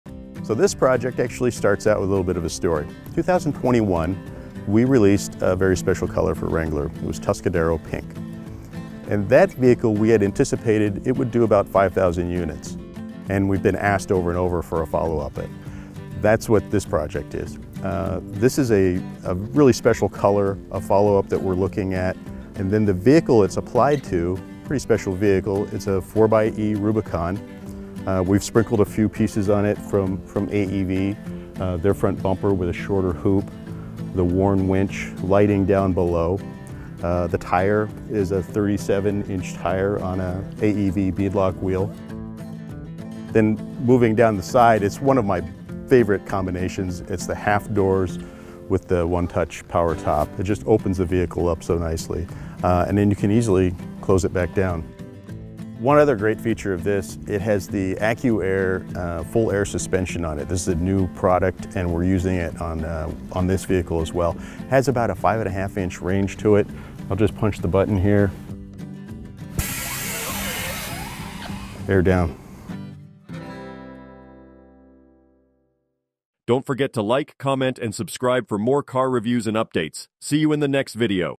2023 Jeep Wrangler Rubicon 4xe | Easter Jeep Safari
The Jeep Wrangler has always been known for its rugged and adventurous spirit, inspiring drivers to take on the toughest terrain and embrace the great outdoors. But in 2021, Jeep took things to the next level with the special-run, limited-edition Tuscadero exterior paint color. This bold and vibrant shade of magenta turned heads and captured imaginations, becoming an instant hit with Jeep fans and car enthusiasts alike.

Buoyed by the overwhelmingly positive response to the Tuscadero paint color, the Jeep design team was inspired to take things even further. They wanted to push the boundaries of what was possible with this head-turning hue, and make it even tougher and more rugged than before.

So the team drew a line in the sand – quite literally, in fact, as they convened in the rugged Moab desert to begin their work. There, surrounded by towering rock formations and challenging off-road terrain, they set to work on reimagining the Tuscadero color.

The result was a new, tougher version of Tuscadero that retained the vibrant and eye-catching hue of the original, but added a touch of grit and determination that was unmistakably Jeep. This new version of Tuscadero was designed to stand up to the toughest conditions, and to embody the adventurous and fearless spirit of the Jeep brand.

With this new color, Jeep fans can show off their love of adventure and their willingness to take on any challenge. Whether cruising down the highway or exploring the backcountry, the tough and resilient Tuscadero color is sure to turn heads and inspire envy in anyone who sees it.

So if you're looking for a way to make a statement and stand out from the crowd, consider adding the limited-edition Tuscadero color to your Jeep Wrangler. With its unbeatable combination of bold style and rugged durability, it's sure to be a hit wherever you go.